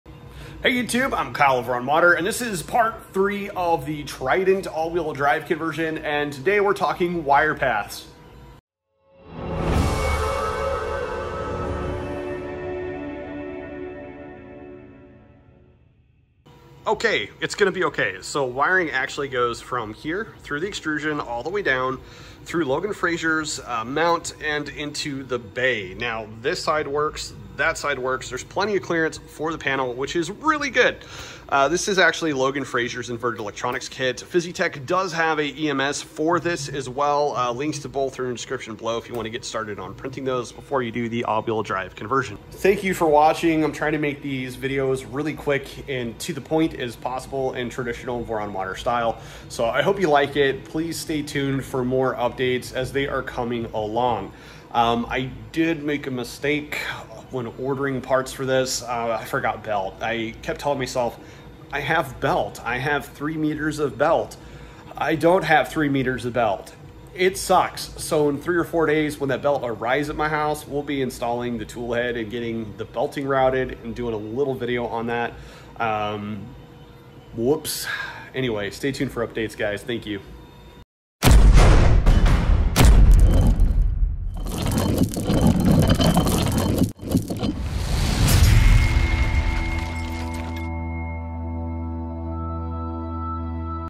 Trident AWD Wiring Fix Part 4
I figured out decent wiring option here.